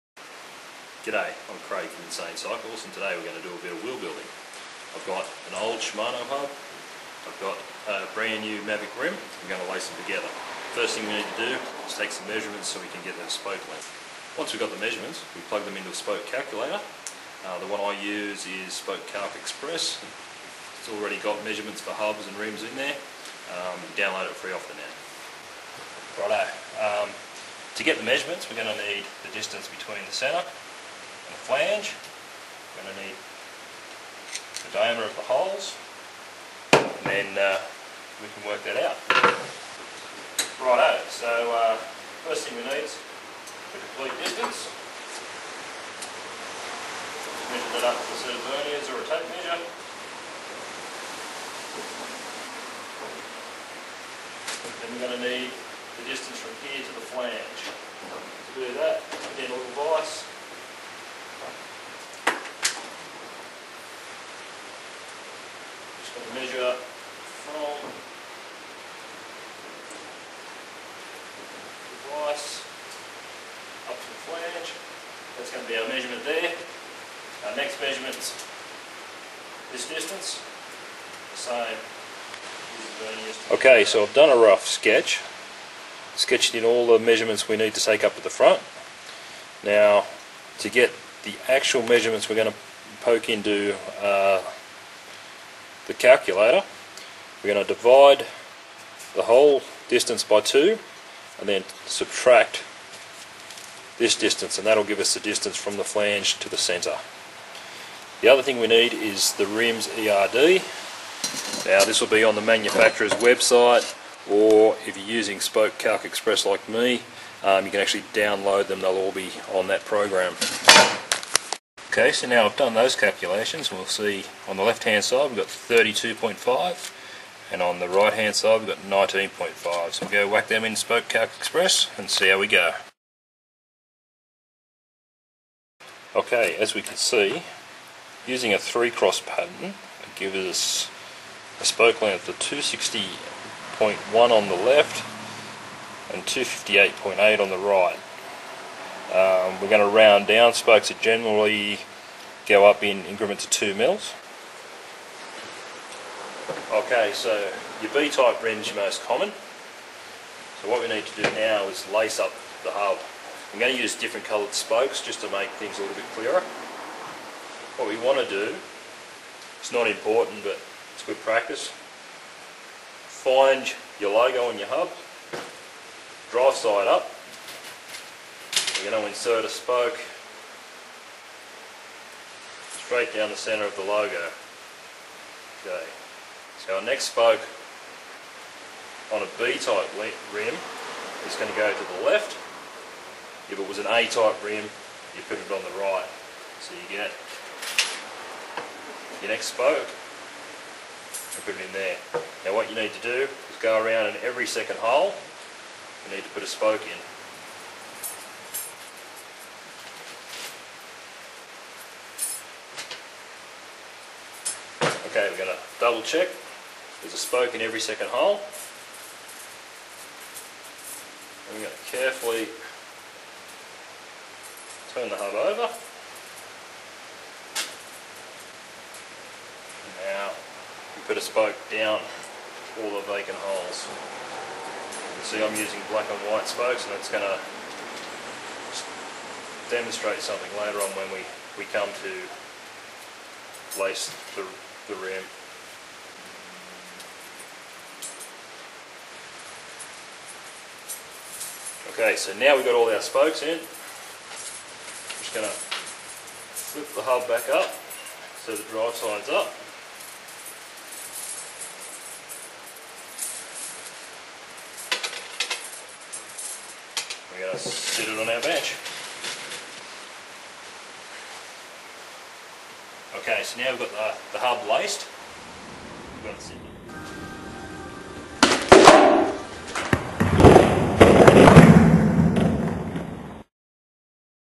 wheelbuilding1
How to build a bicycle Wheel Part1: Working out Spoke Lengths and lacing the hub